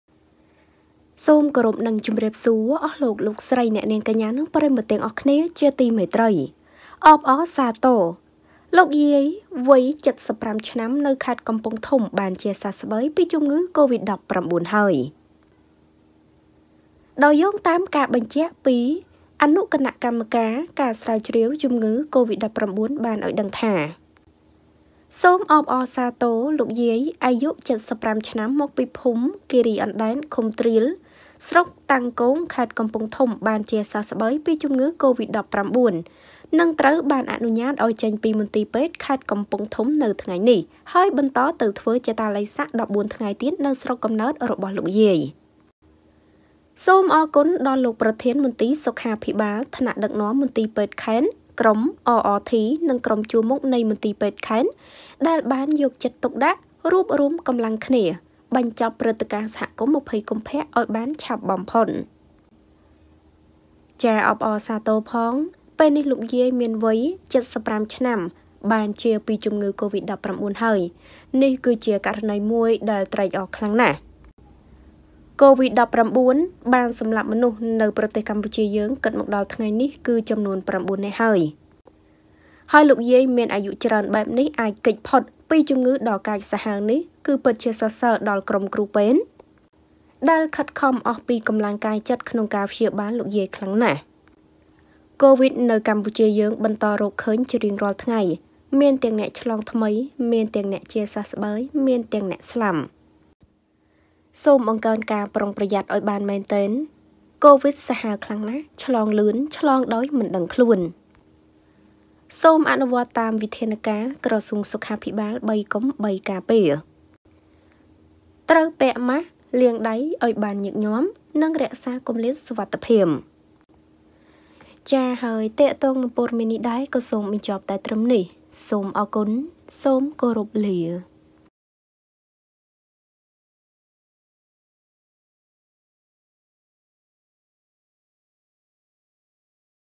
អាយុថ្លៃ! អបអរសាទរ លោកយាយអាយុ៧៥ឆ្នាំនៅខេត្តកំពង់ធំ បានជាសះស្បើយពីជំងឺកូវីដ១៩ហើយ
សូមអធ្យាស្រ័យ

ចំពោះការមិនបានតប ខំមិនបានគ្រប់គ្នា

Ms Socheata